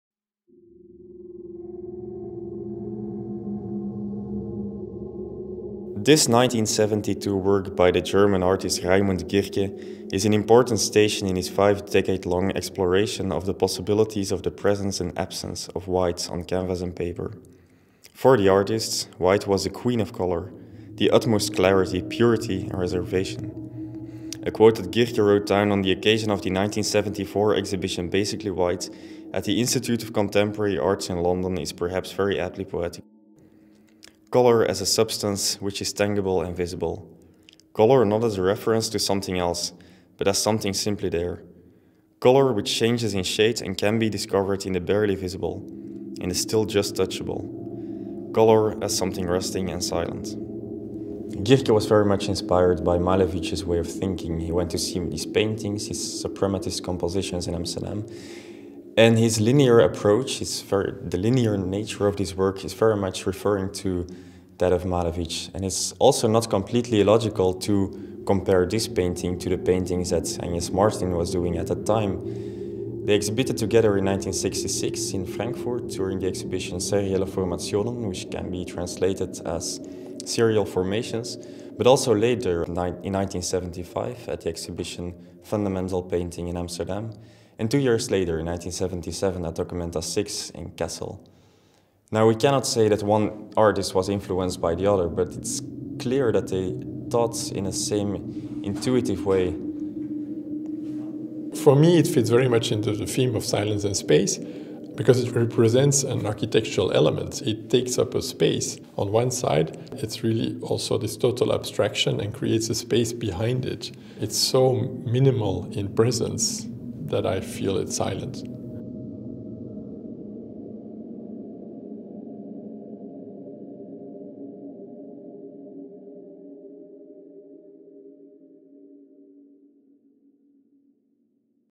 Raimund Girke: Exhibition Silence & Space at Axel Vervoordt, Kanaal
Sander Bortier presents Untitled work by Raimund Girke, featured in a group exhibition titled 'Silence & Space' at Axel Vervoordt, Kanaal, Wijnegem.